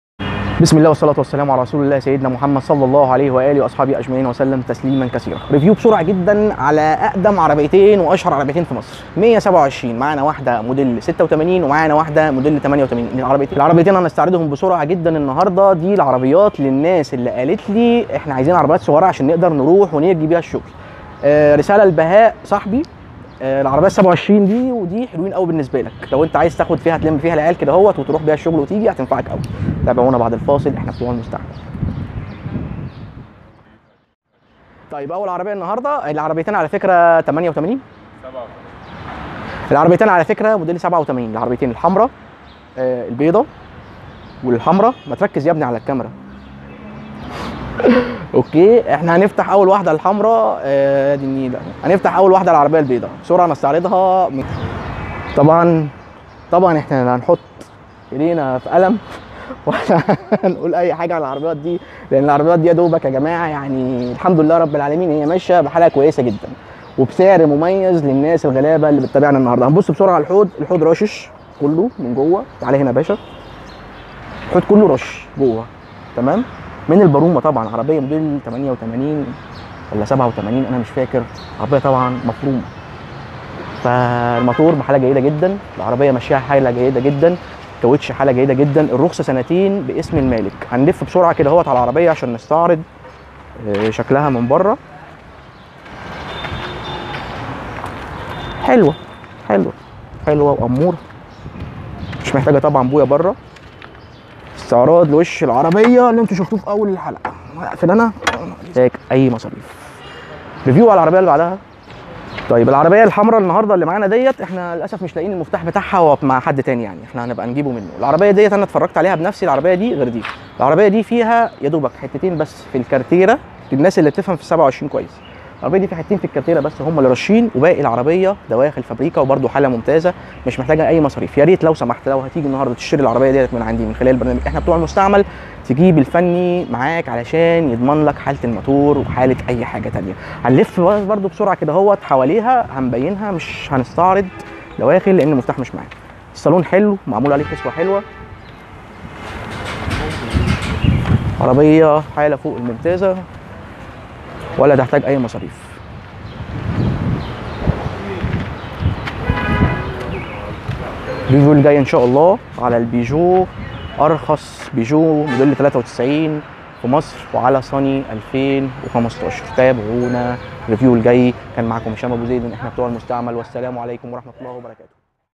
أرخص عربيتين للبيع في مصر فيات ١٢٧ فيورا عربية بتمن موبيل احنا بتوع الغلابة😊🚗 sold 👍
كيف_تشترى_عربية_مستعملة 👇 جزء 1

جزء ٢